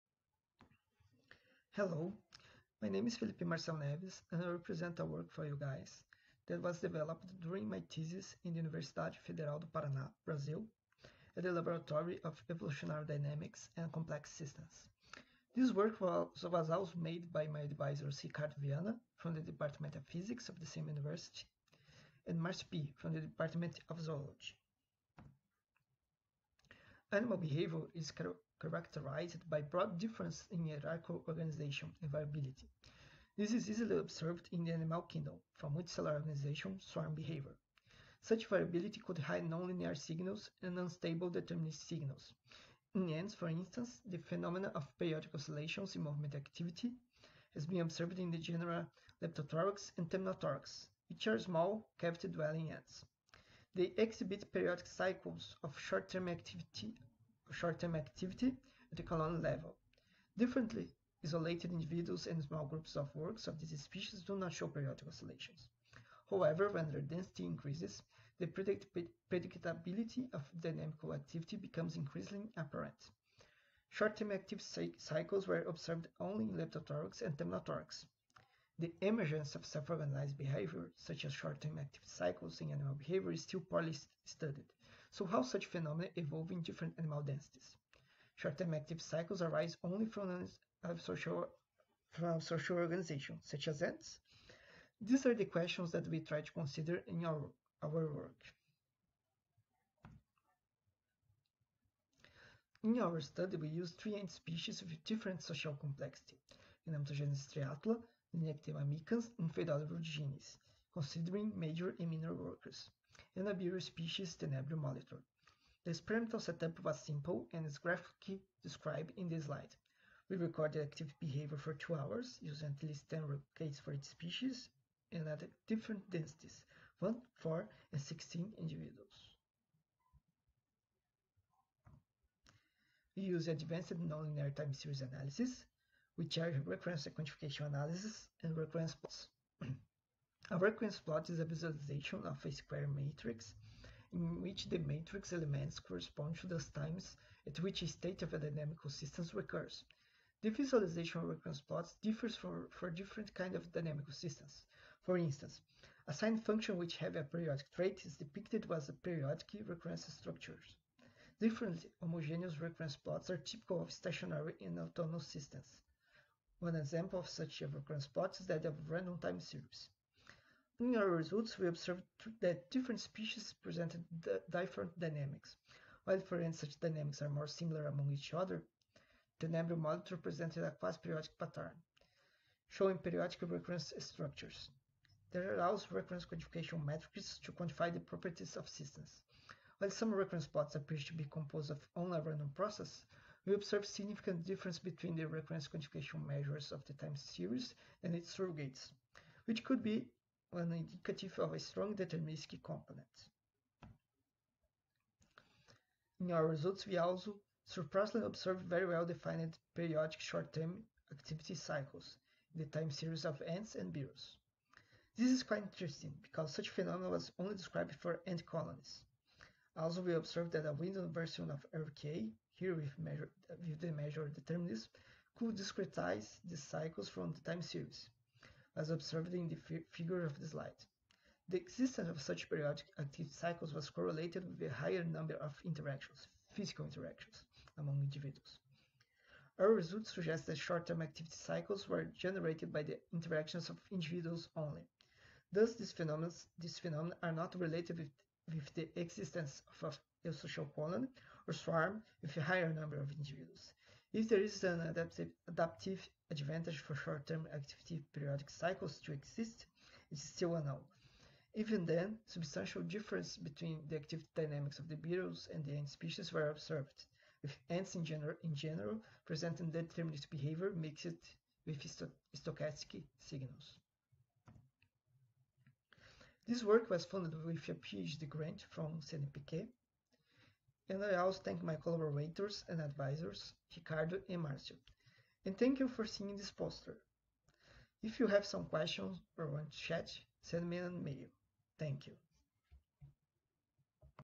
Virtual poster n°66 - Felipe Marcel Neves
Title: "Recurrence analysis of ant activity behavior in different densities"

Authors: Felipe M. Neves (1), Ricardo L. Viana (1), Marcio R. Pie (1)

(1) Universidade Federal do Paraná

Abstract: "The phenomenon of periodic oscillations in movement activity has been observed in ants of the genera Leptothorax and Temnothorax (small, cavity-dwelling ants), which exhibit periodic pulses of short-term activity at the colony level. Isolated individuals and small groups of workers of these species do not show periodic oscillations, but when their density increases, the predictability of dynamic activity becomes increasingly apparent. Although these results are intriguing, they have never been observed in other ant genera, nor has the transition to periodicity been well studied. Here we used recurrence quantification analysis (RQA) and recurrence plots (RPs) to compare and analyze the activity dynamics of ant species with different levels of social complexity and at different densities (1, 4 and 16 individuals). RQA and RPs are powerful nonlinear data analysis methods that quantify the number and duration of the phase space trajectory of the dynamical system, resulting in a detailed description of its behavior, including the quantification of dynamic patterns (stochastic to deterministic). Three species of ants were used in our study: Gnamptogenys striatula (Ectatomminae), Linepithema micans (Dolichoderinae), Pheidole rudigenis (Myrmicinae), as well as adults of a gregarious beetle species (Tenebrio molitor) for comparison. The local interactions among ant workers could explain the emergence of the oscillatory activity, thus we also quantify the frequency of the interactions between ant workers. Our results (i.e. 150 time series; 300 hours) show that the activity dynamics of ant groups are more predictable in comparison with isolated individuals and that periodic oscillations are caused by a higher number of local interactions, as expected. Interestingly, not only some ant species, but also beetles exhibit characteristic short-term periodic activity pulses in small groups of individuals, suggesting that such phenomena could be a by-product of the contact rate between individuals rather than an innate phenomenon of ant societies."